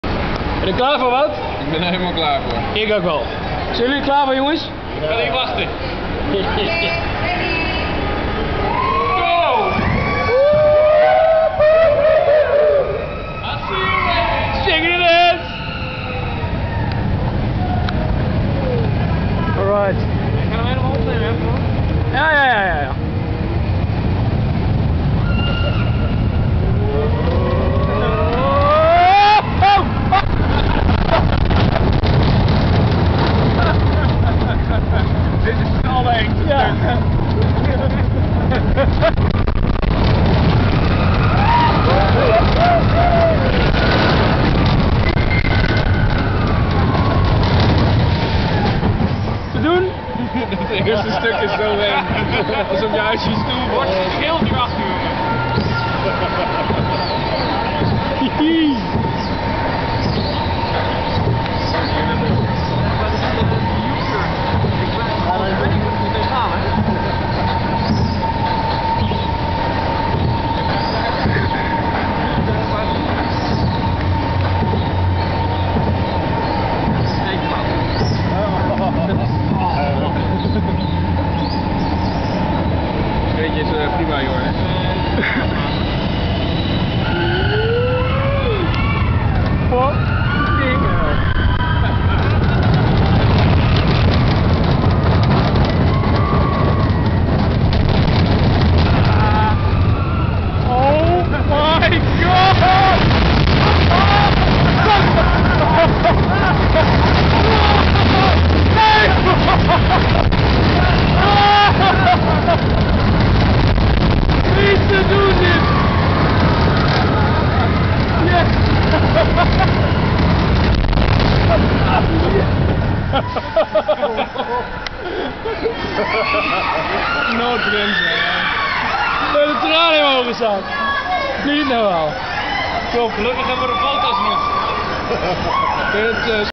Indoor rollercoaster Kuala Lumpur (Times Square)
A ride with the indoor rollercoaster inside mall Times Square in Kuala Lumpur, Malaysia.